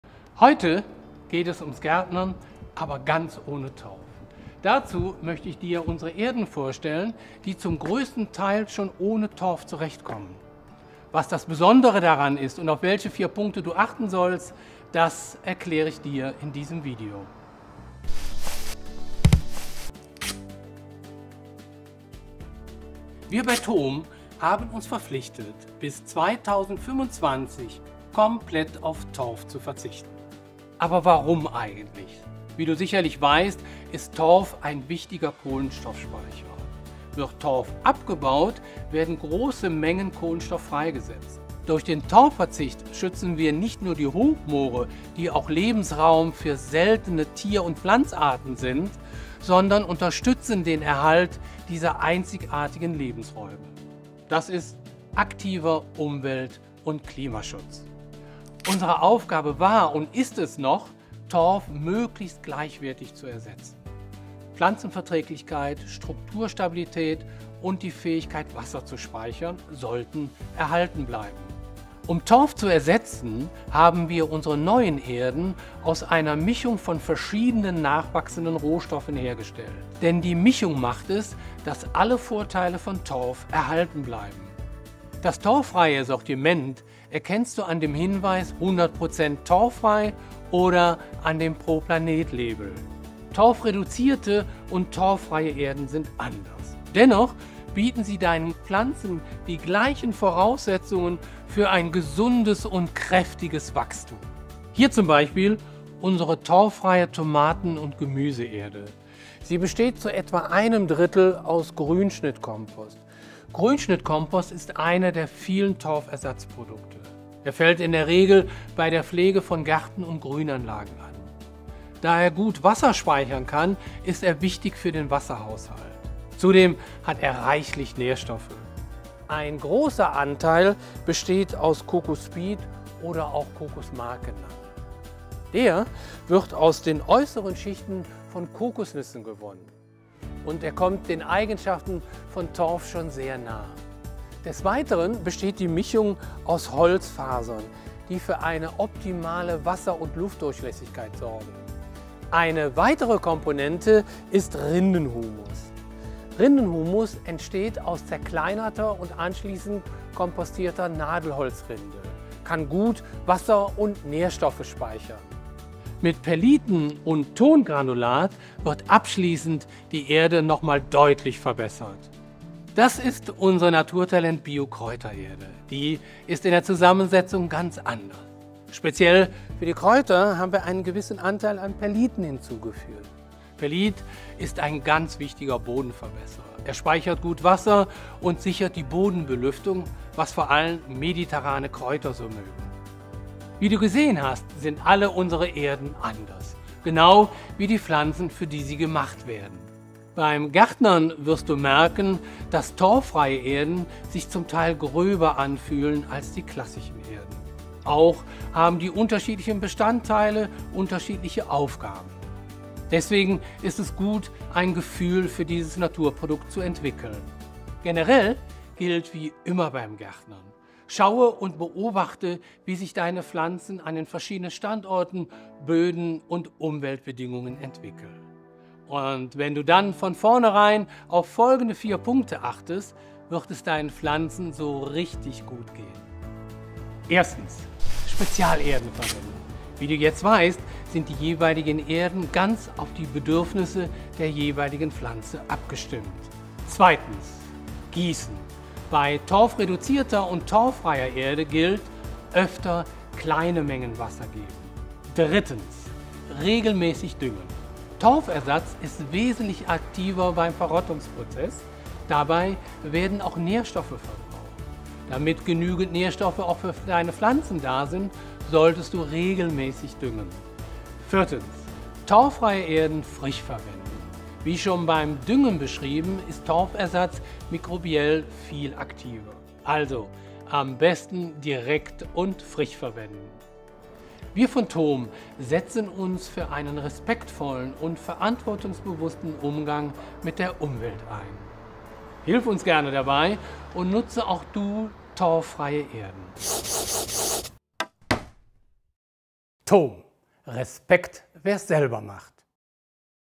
toom Pflanzentipps: Torffreie Erden | toom Baumarkt
Wusstest du, dass unsere Erden schon zum größten Teil ohne Torf zurecht kommen? Warum das besonders ist & auf welche Punkte du außerdem achten sollst erfährst du in diesem Video.

toom – Respekt, wer’s selber macht